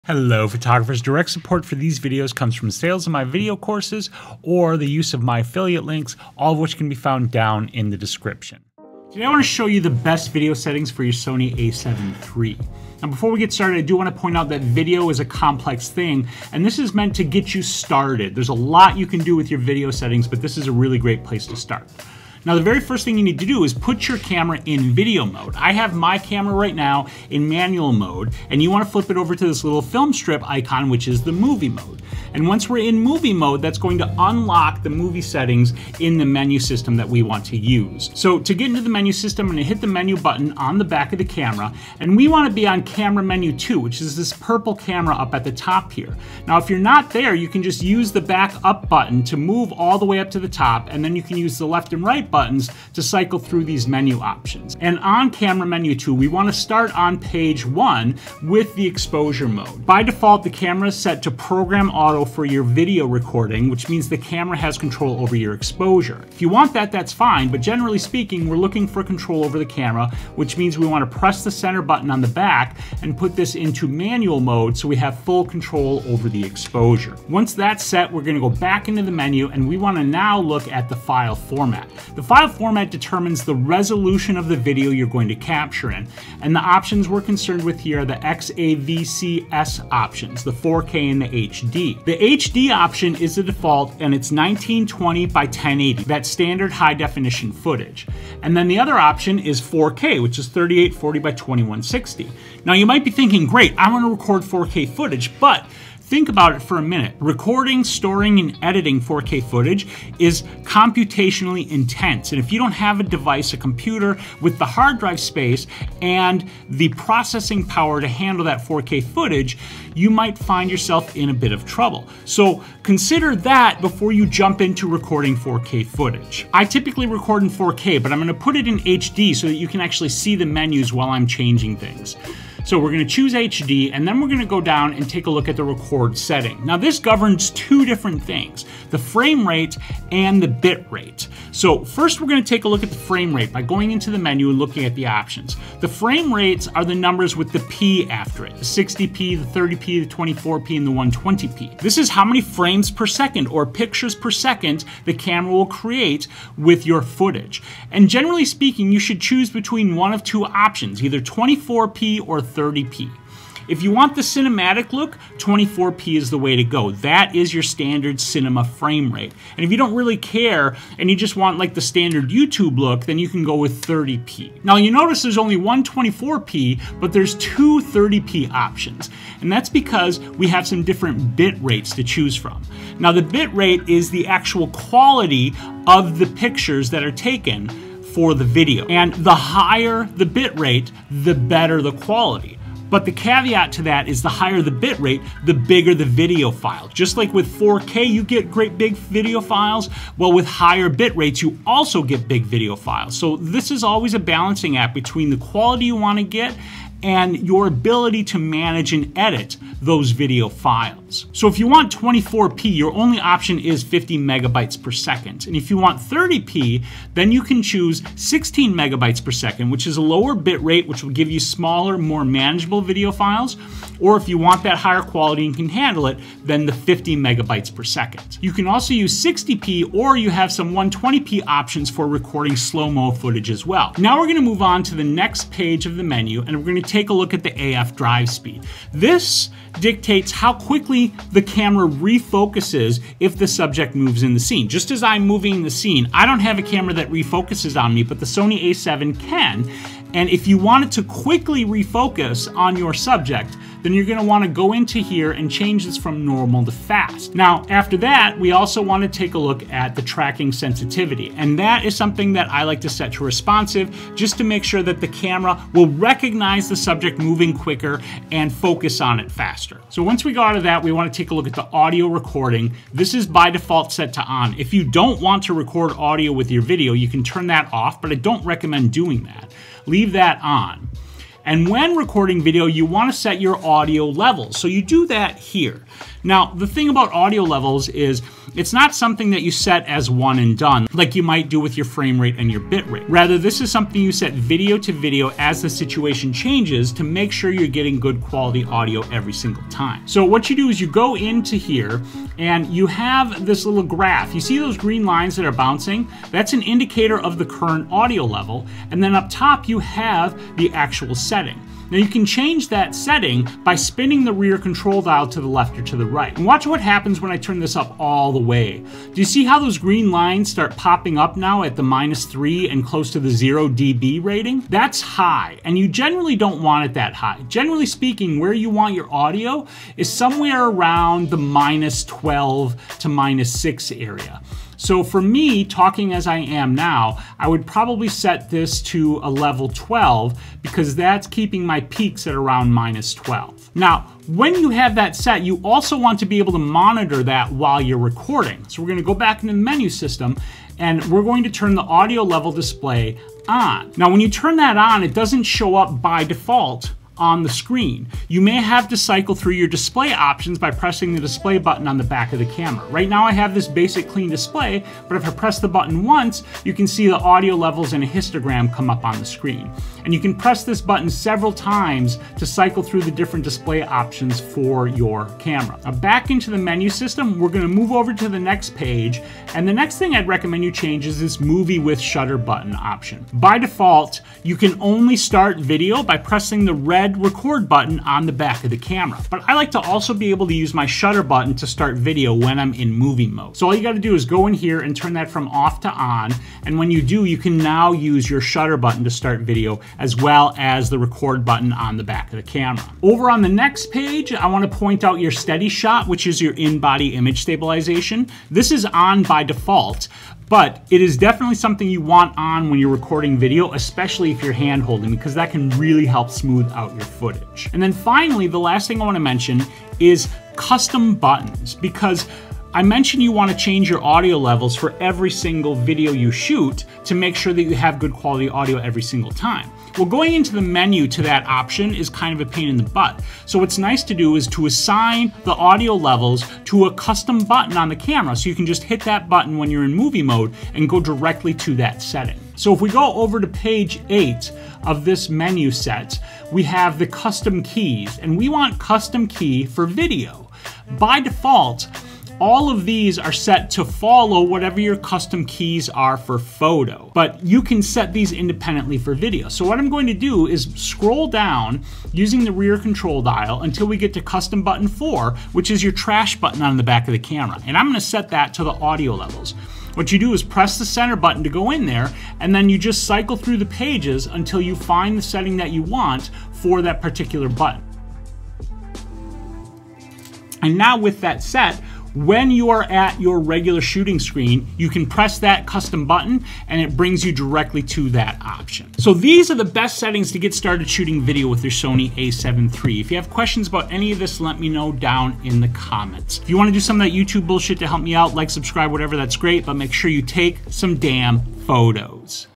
Sony A7III Best Video Settings
In this video I explain the best video settings for the Sony A7III to get you up and running shooting video.